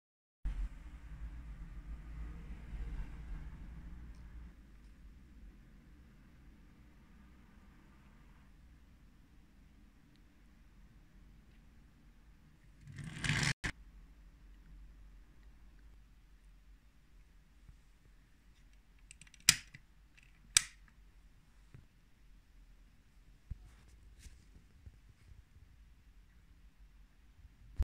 Ford Mustang from 2015 Scale 1/34 Diecast Model RMZ City: The 50TH Anniversary Mustang!
Hello! Here are some informations about this 50TH Anniversary:
Why is the 2015 Mustang the 50th anniversary? Ford first introduced the Mustang as a 1964 1/2 model, and it was an instant success. In order to celebrate the car's 50th (and 1/2) anniversary, Ford is releasing the 2015 Mustang 50 Year Limited Edition.

SOURCE: Google